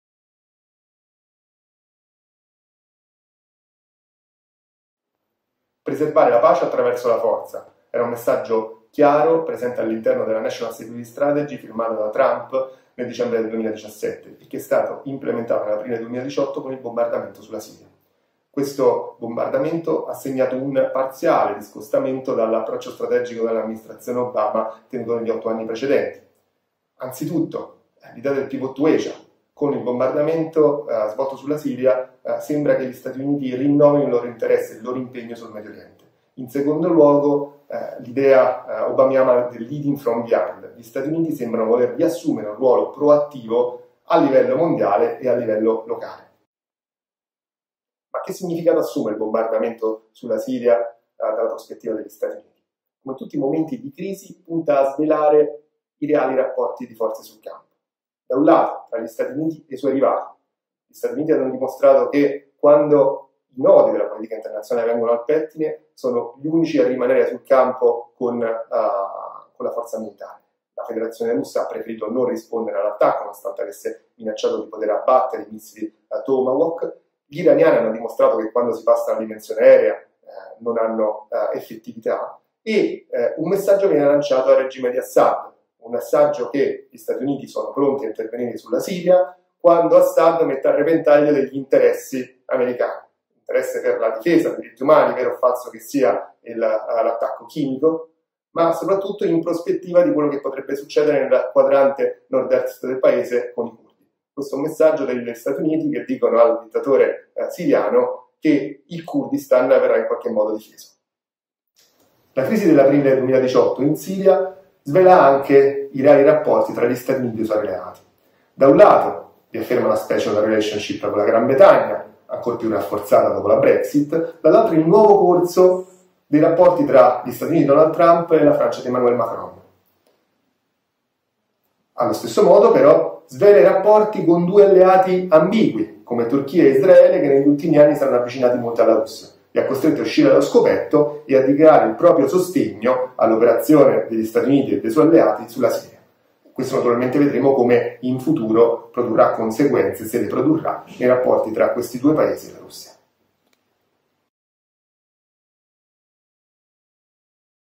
G Natalizia - Le ragioni dell attacco in Siria
Il commento di Gabriele Natalizia, coordinatore di Geopolitica.info, sullo strike contro la Siria compiuto da USA, Francia e Gran Bretagna.